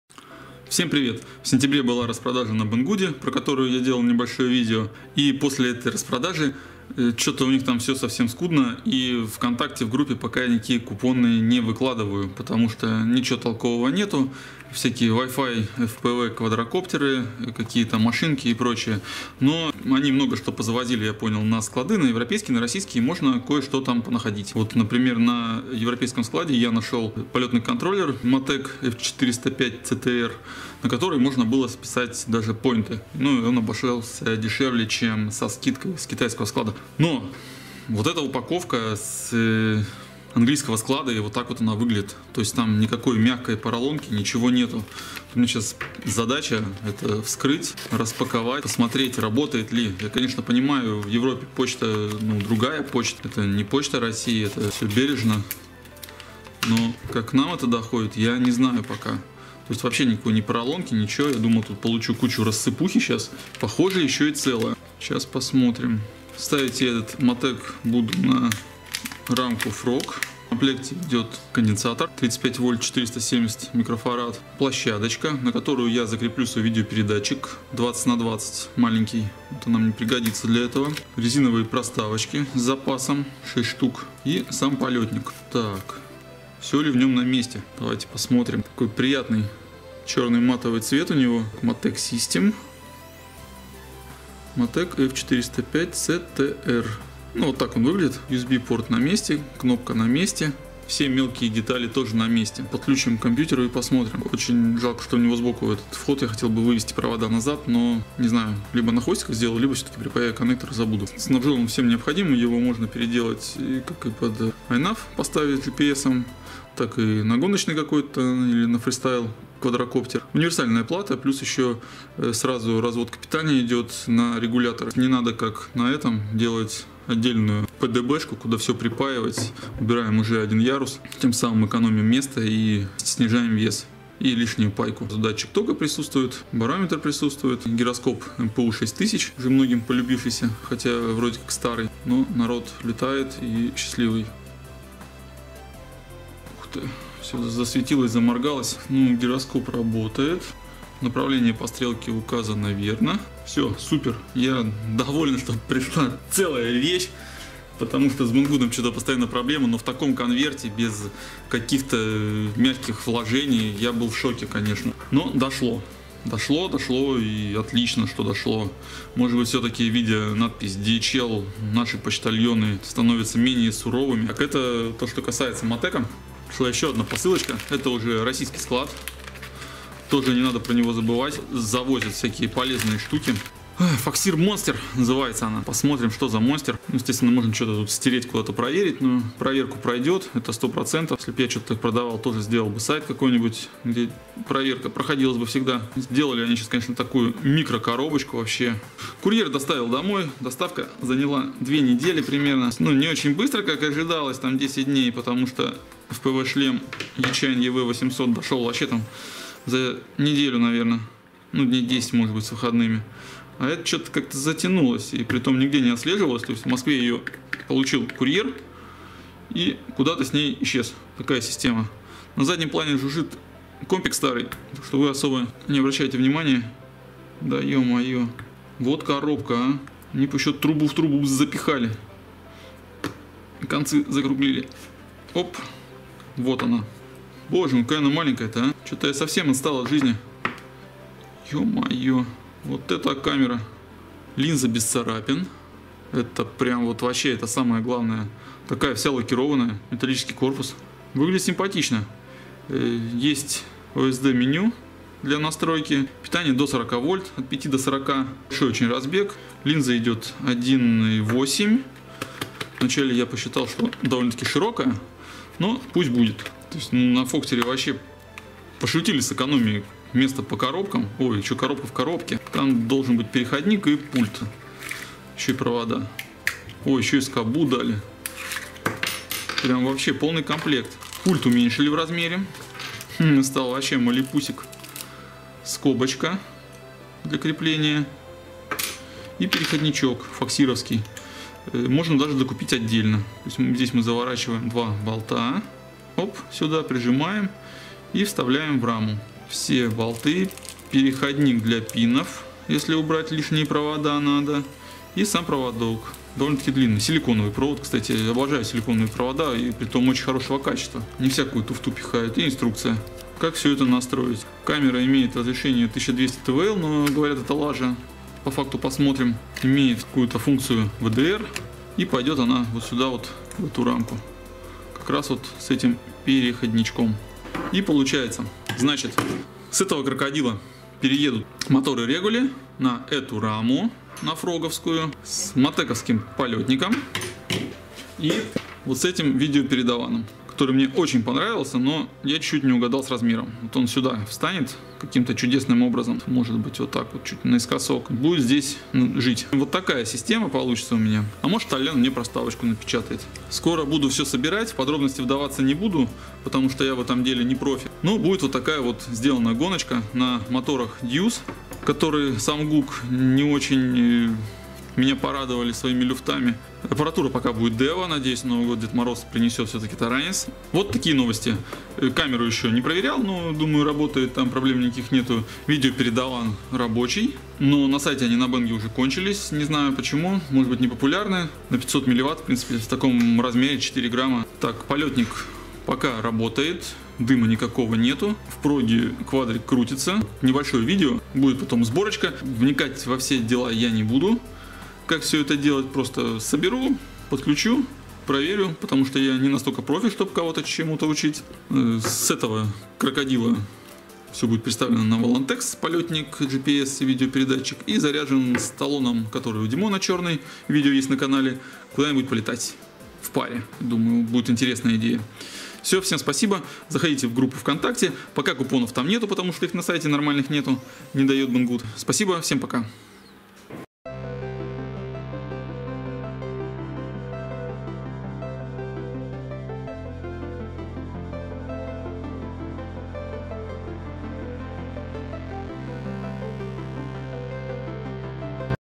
Matek F405-CTR и FPV камера Foxeer Monster Mini Pro на раму TransTEC Frog Lite
Полетный контроллер Matek Systems BetaFlight F405-CTR
-8$. Итого 31.99$ (~2077 руб.) до 07.10.19. Купон: BGCTR101

Foxeer Monster Mini Pro 1200TVL
Прочее:
FPV видеопередатчик 30х30 Speedy Bee VTX-DVR 5.8G 48CH 600mW
TransTEC Frog Lite комплект популярной рамы.

RC POiNT